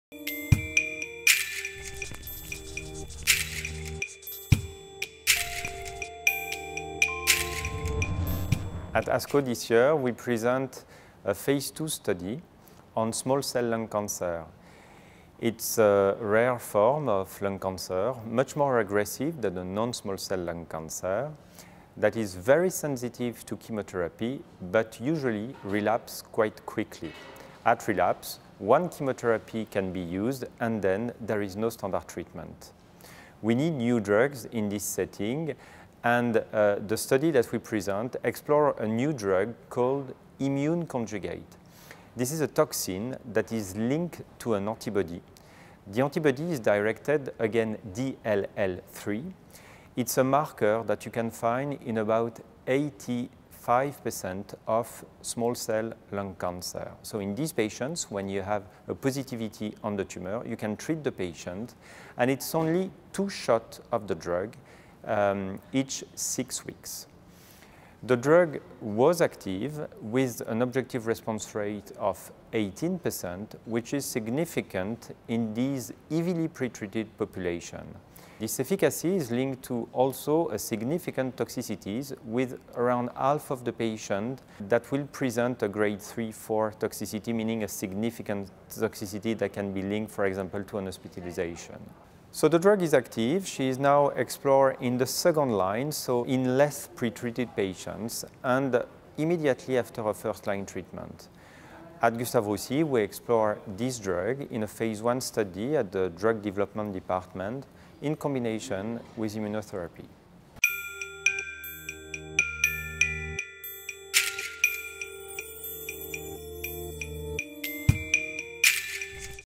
Efficacy of a new treatment in small cell lung cancer
Prof. Benjamin Besse, Head of medical oncology department at Gustave Roussy, presents at ASCO 2018 the results of the TRINITY study, a phase II trial evaluating rovalpituzumab tesirine in patients with small cell lung cancer DLL3-expressing.